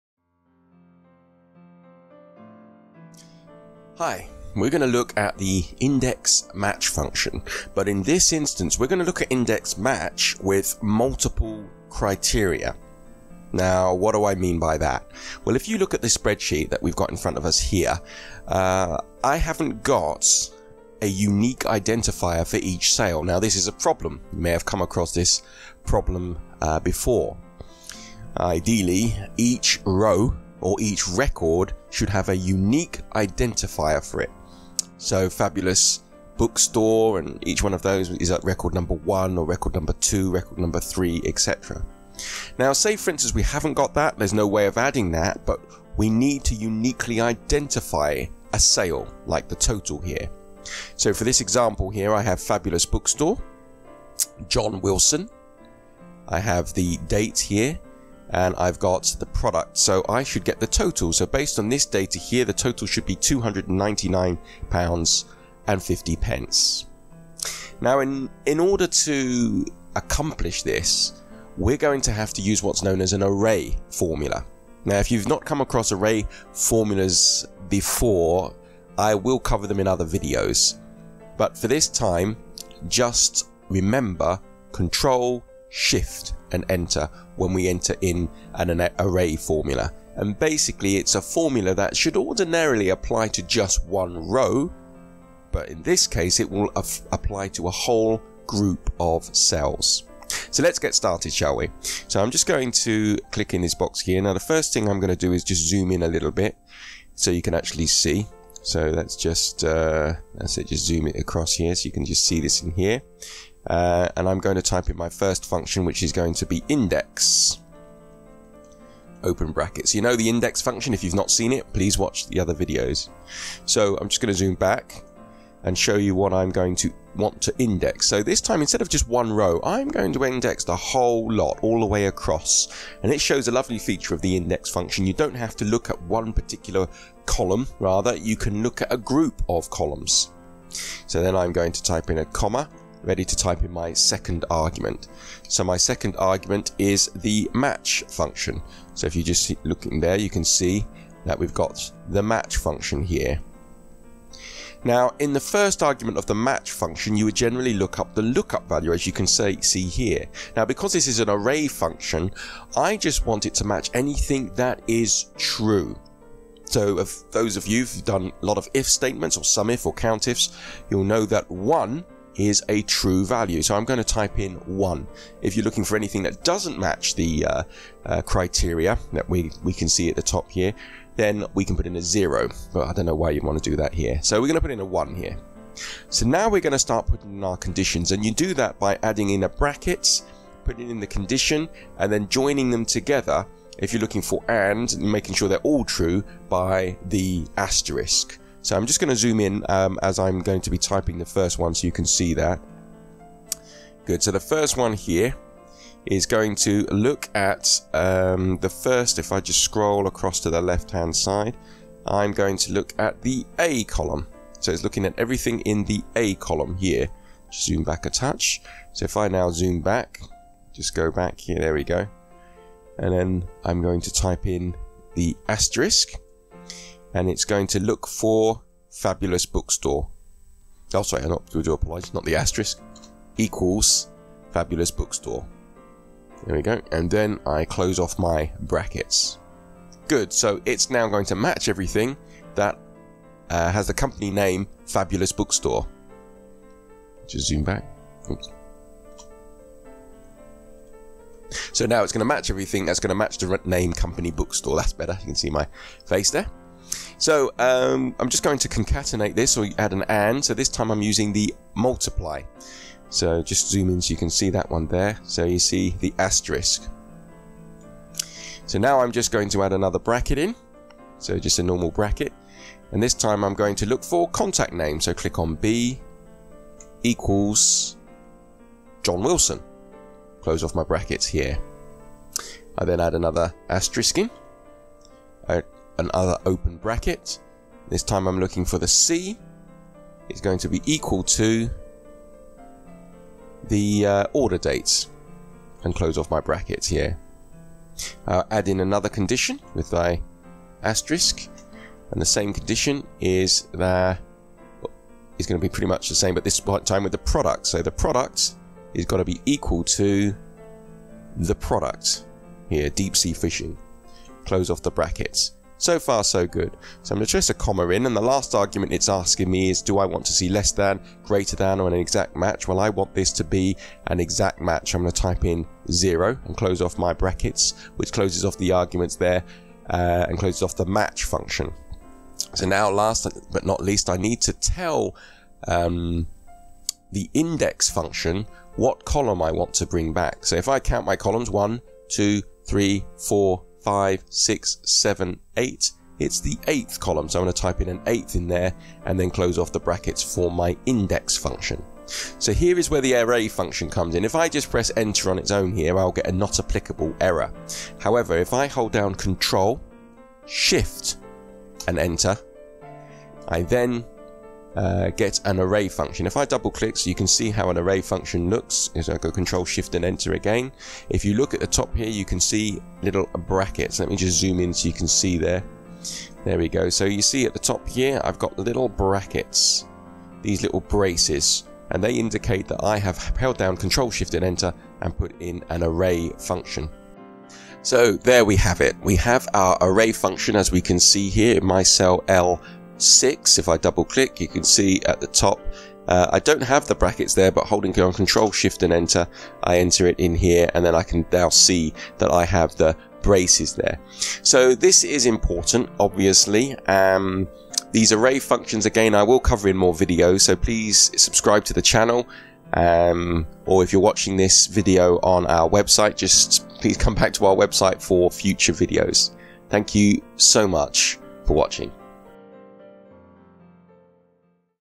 Index Match Multiple Criteria
This tutorial will show you Index Match multiple criteria, what does that mean? Well, you no doubt have progressed onto the Index Match function from VLookup in most of your formulas. What though if you don't have a unique identifier for each row in your range? If that is the case you can follow this tutorial to help you.

In this tutorial if the Company Name, Contact Name, Order Date and Product are all the same then we are going to assume that this is a unique row and hence we can use this row to match data in another table.

This tutorial will teach you how to return just a single value, but if you would like to return multiple values please check out

Using Index Match with multiple criteria is possible in Excel. Especially when you need to uniquely identify a row without an ID column as in this example.

Fancy an in person training course? Check us out